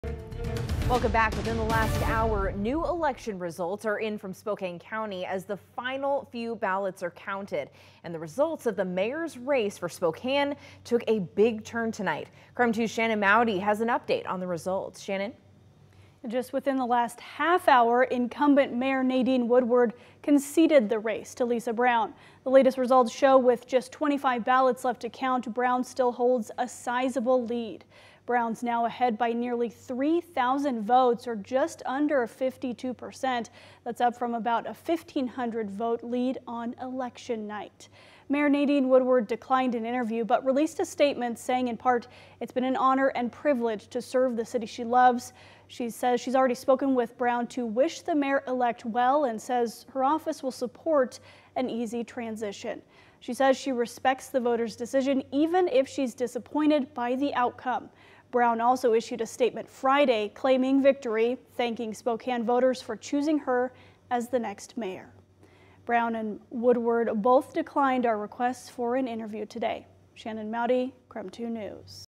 Nadine Woodward concedes in race for Spokane Mayor
In a new statement, the former mayor of Spokane says she will leave office "in a few short weeks."